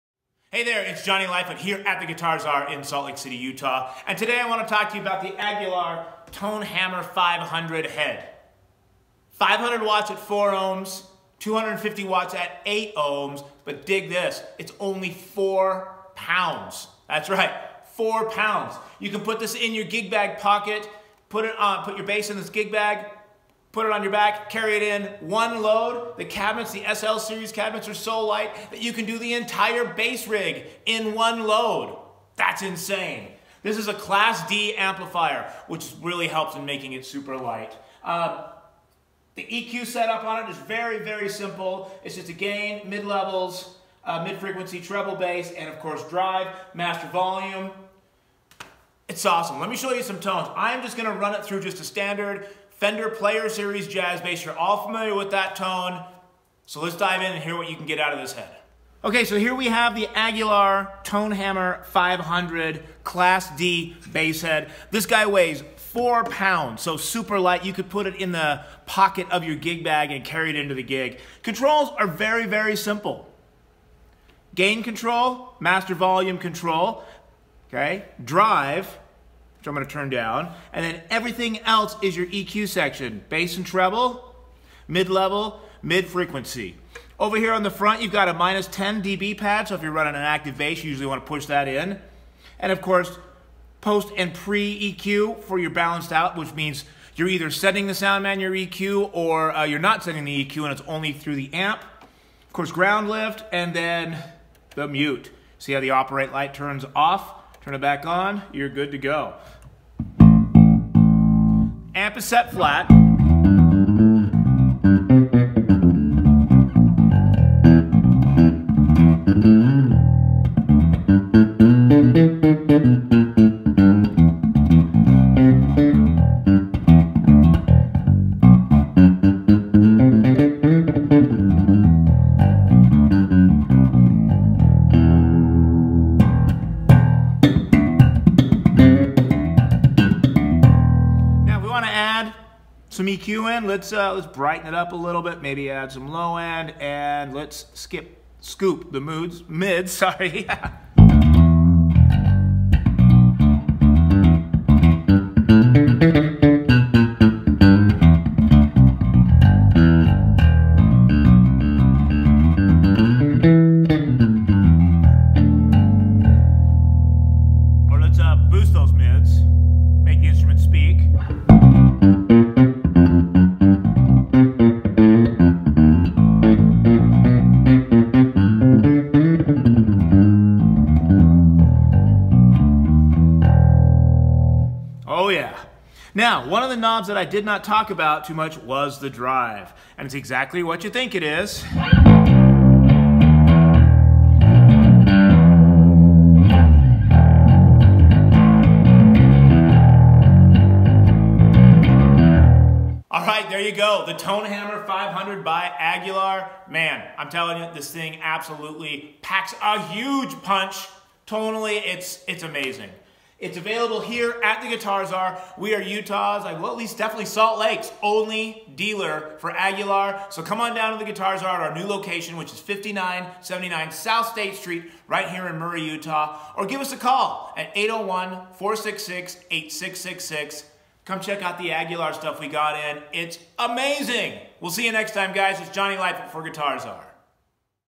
Guitar Czar Aguilar Tone Hammer 500 Run Through
Jonni Lightfoot runs through the Aguilar Tone Hammer 500 bass head.Super light weight weighs 4 pounds but packs a HUGE punch!